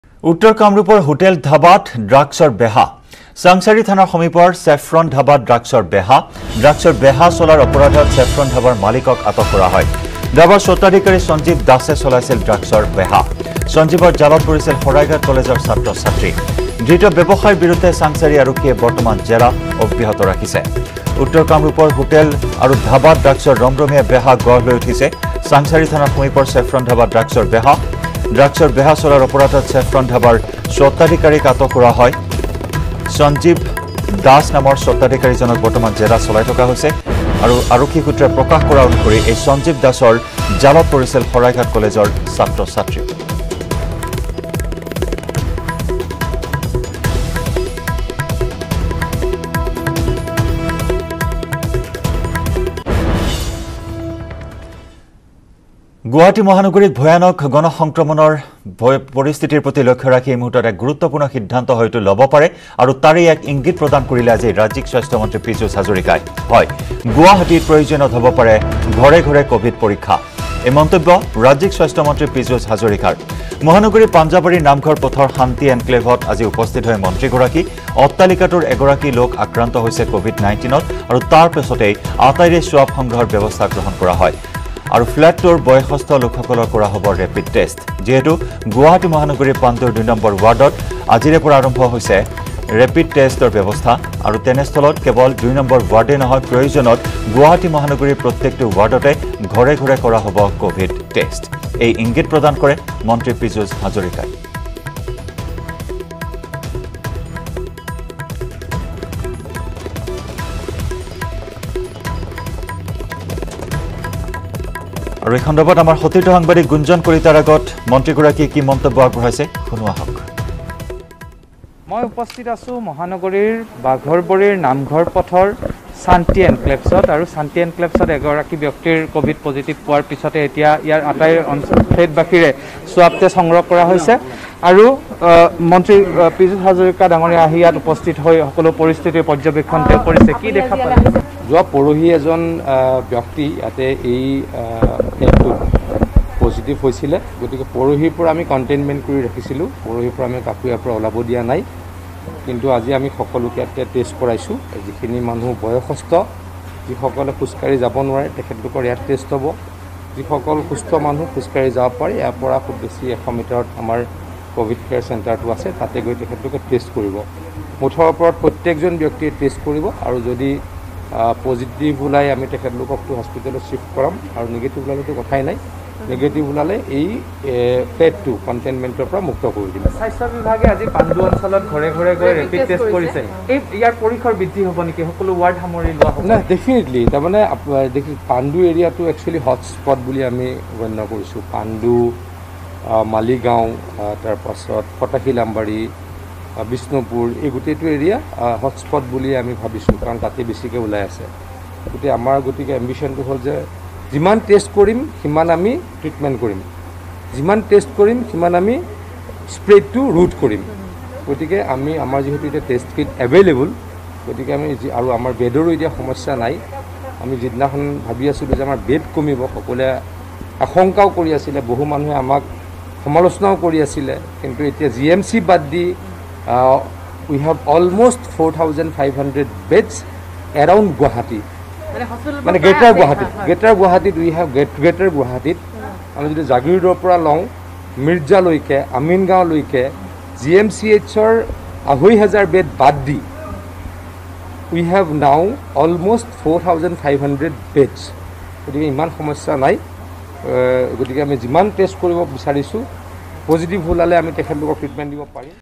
উত্তৰ কামৰূপৰ হোটেলত Drugsৰ বেহা, আটক ধাবাৰ মালিকক
চাংসাৰী থানাৰ সমীপৰ চেফ্ৰন ধাবাত Drugsৰ বেহা। ধাবাৰ স্বতাধিকাৰী সঞ্জীৱ দাসে চলাইছিল ড্ৰাগছৰ বেহা।